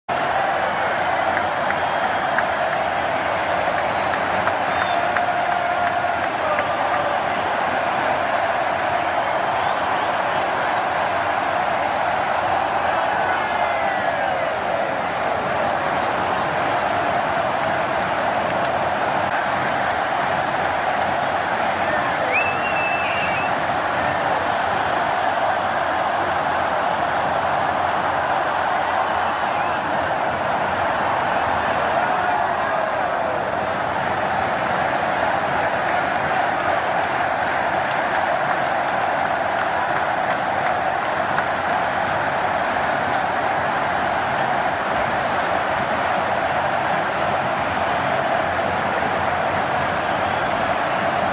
Flamengo 2 x 0 JEC Torcida adversária na Ola
Flamengo vencendo por 2 a 0 e a torcida do JEC fazendo ola junto. Aula de paz e civilidade!